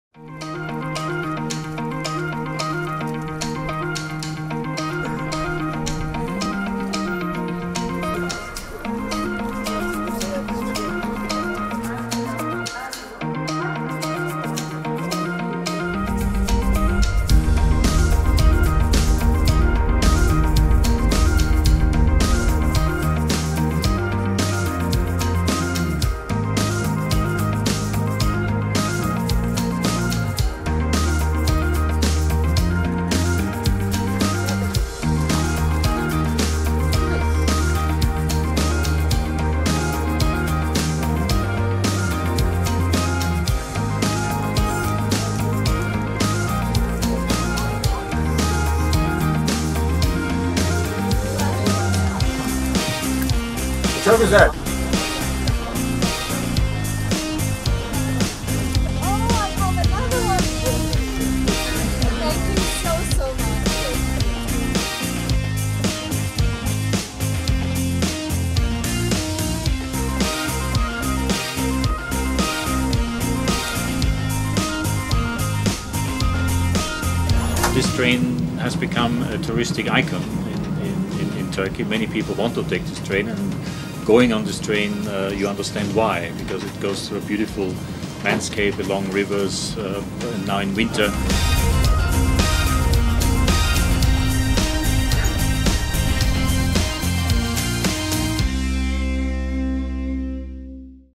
EU-Botschafter im Orient Express
Vertreter der Europäischen Union reisen mit dem Orient-Express ins Morgenland: Christian Berger, EU-Botschafter der Türkei, und weitere Delegierte der EU haben sich mit dem Expresszug auf eine ereignisreiche Fahrt von Ankara bis nach Kars begeben.